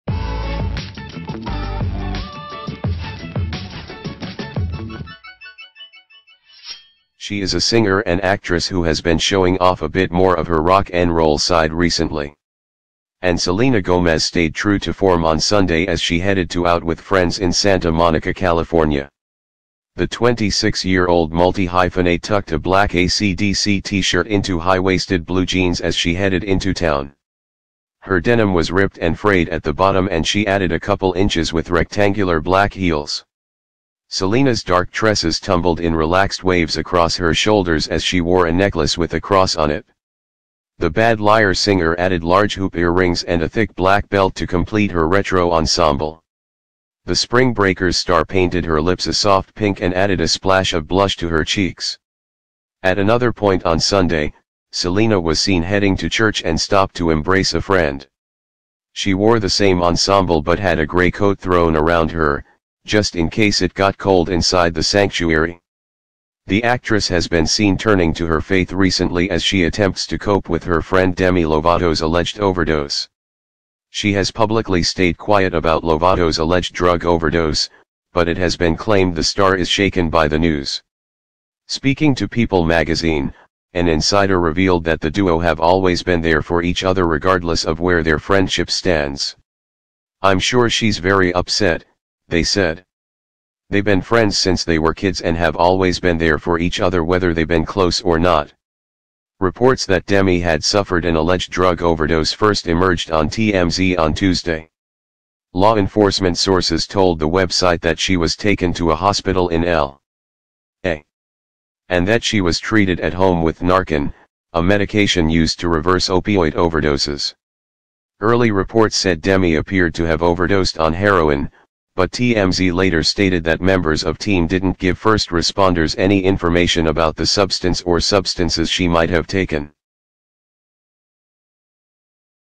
Selena Gomez with friends in Santa Monica, California
Selena Gomez cuts a casual figure in high-waisted blue jeans and AC/DC T-shirt in Santa Monica.